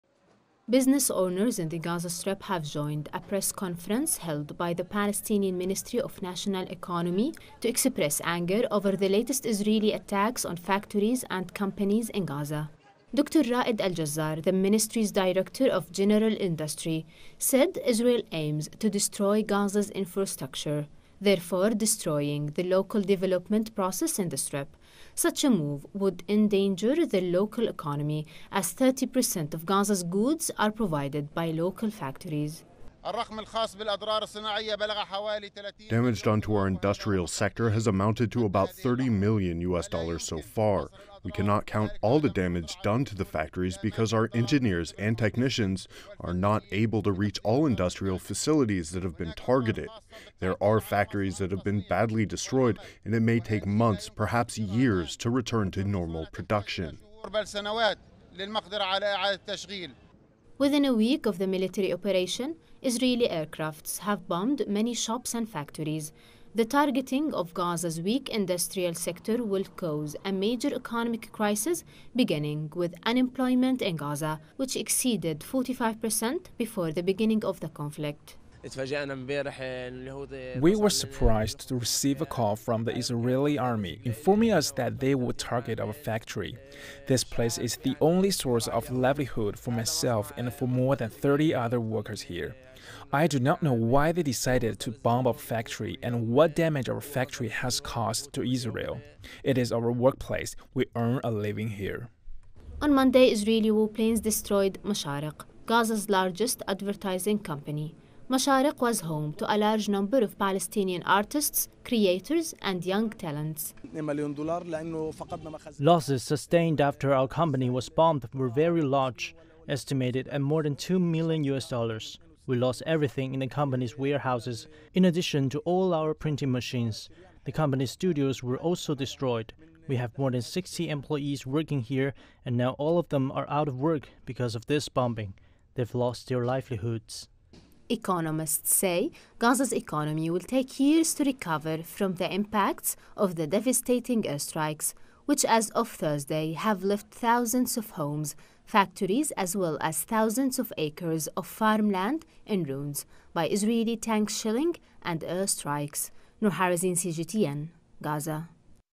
Gaza reports huge damage to industrial sector
Gaza's ministry of the economy reported significant damage to the region's industrial sector amid the deadly conflict.